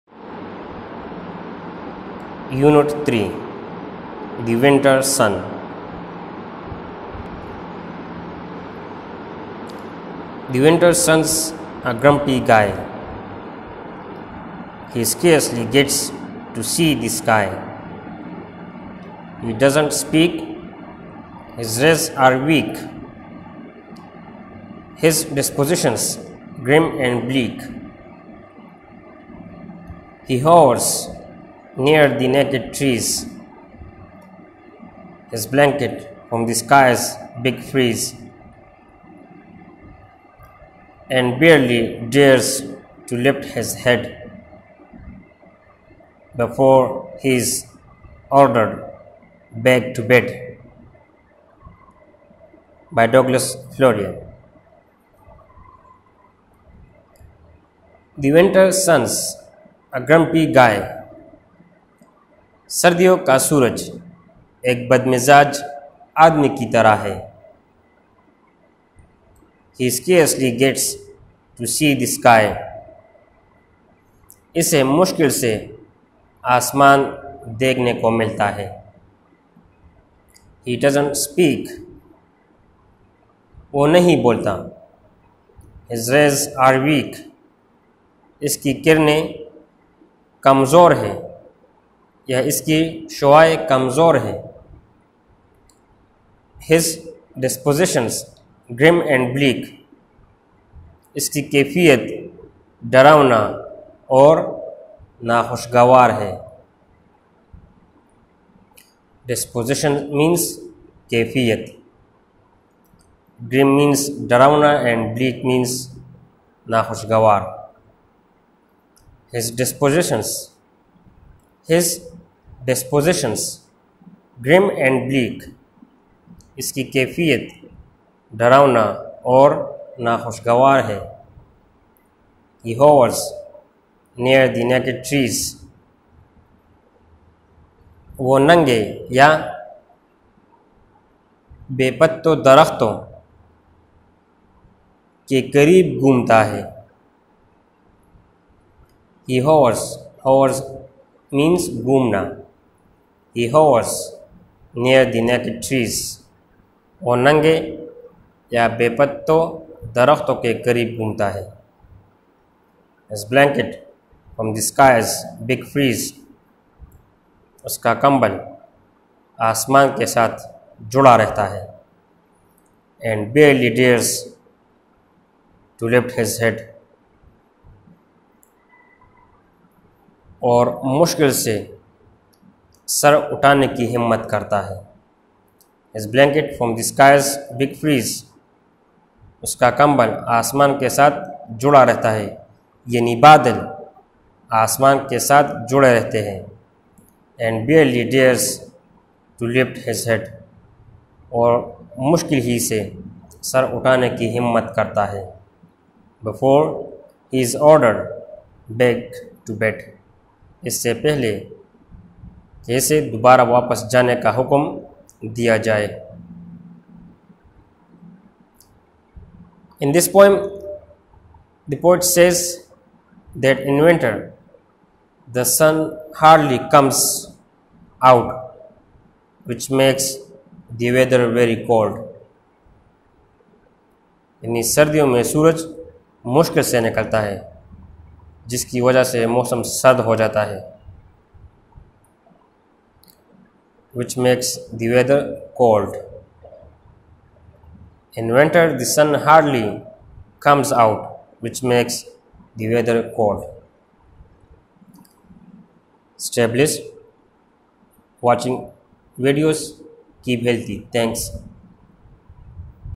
CANTAB# ENGLISH# CLASS 7# UNIT 3# LECTURE 1# POEM# THE WINTER SUN
In this poem, the poet says that in winter the sun hardly comes out, which makes the weather cold. With figurative language, the feelings of the sun are expressed.